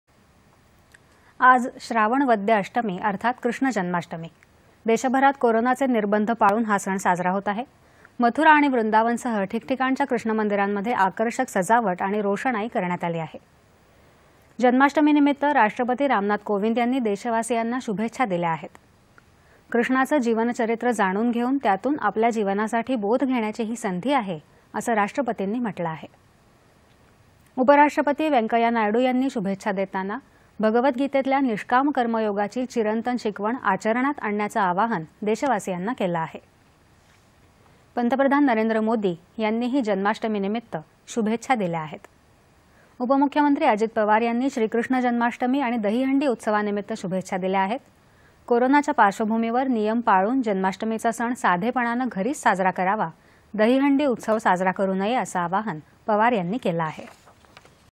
देशभरात कृष्णजन्माष्टमी उत्सव
३०.०८.२०२१ :
 आज श्रावण वद्य अष्टमी अर्थात कृष्णजन्माष्टमी. देशभरात कोरोनाचे निर्बंध पाळून हा सण साजरा होत आहे. मथुरा आणि वृंदावनसह ठिकठिकाणच्या कृष्णमंदिरांमध्ये आकर्षक सजावट आणि रोषणाई करण्यात आली आहे.
   जन्माष्टमी निमित्त राष्ट्रपती रामनाथ कोविंद यांनी देशवासियांना शुभेच्छा दिल्या आहेत. कृष्णाचं जीवनचरित्र जाणून घेऊन त्यातून आपल्या जीवनासाठी बोध घेण्याची ही संधी आहे, असं राष्ट्रपतींनी म्हटलं आहे.
  उपराष्ट्रपती वेंकैया नायडू यांनी शुभेच्छा देताना, भगवदगीतेतल्या निष्काम कर्मयोगाची चिरंतन शिकवण आचरणात आणण्याचं आवाहन केलं आहे.
पंतप्रधान नरेंद्र मोदी यांनीही जन्माष्टमीनिमित्त शुभेच्छा दिल्या आहेत.
   उपमुख्यमंत्री अजित पवार यांनी श्रीकृष्ण जन्माष्टमी आणि दहिहंडी उत्सावानिमित्त शुभेच्छा दिल्या आहेत. कोरोनाच्या पार्श्वभूमीवर नियम पाळून जन्माष्टमीचा सण साधेपणानं घरीच साजरा करावा, दहिहंडी उत्सव साजरा करु नये, असं आवाहन त्यांनी केलं आहे.